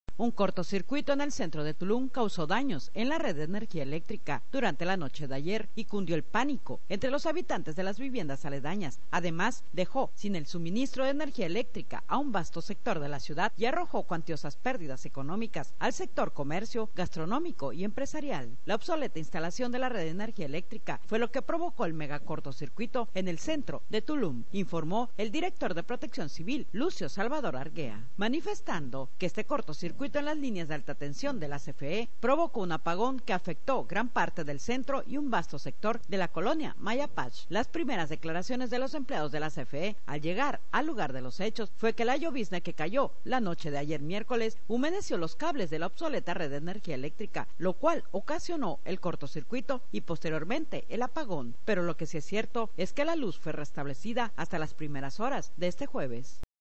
Las lluvias dejan sin luz a Tulum
Un mega cortocircuito, supuestamente por las lluvias de la temporada y no por la obsoleta red de la energía eléctrica, dejo sin luz al centro de Ciudad Tulum y a la colonia Maya Pax. El servicio fue reestablecido hasta que llego el día.